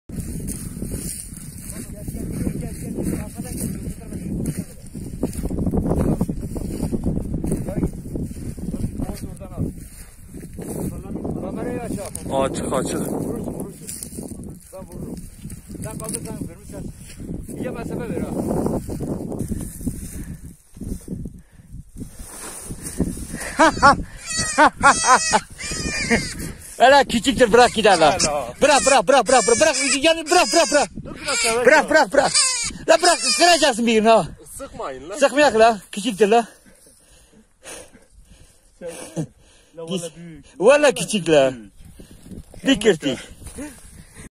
Elleriyle yakaladığı tavşanı geri doğaya saldı
Elazığ'da elleriyle tavşanı yakalayan avcılar, küçük olduğu için tekrar doğaya saldı.